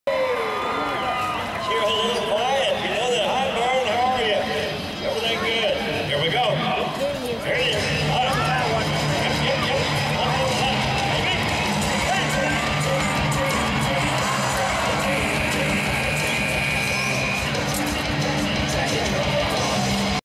Cooper Kanngiesser on the bull 674 All Wound Up.
Bull rider Cooper Kanngiesser gets bucked off Beutler and Son Rodeo's 674 All Wound Up at the 2011 Buffalo Bill Rodeo in North Platte, Neb.